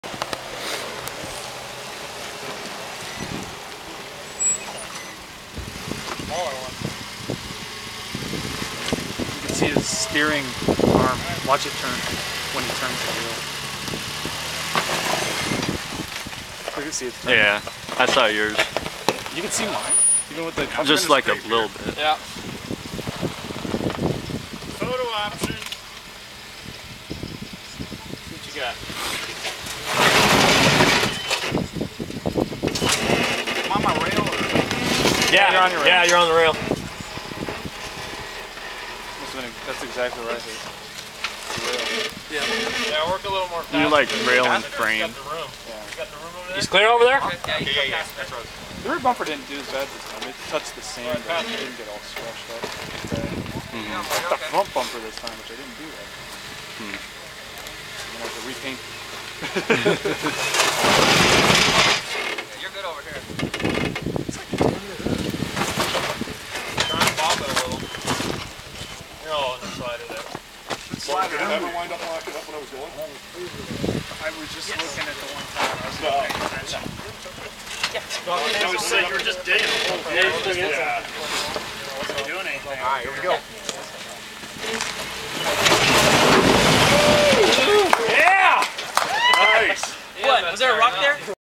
Copper Cache - Jeep Rubicon (Justin)
Justin makes it up the waterfall with ease.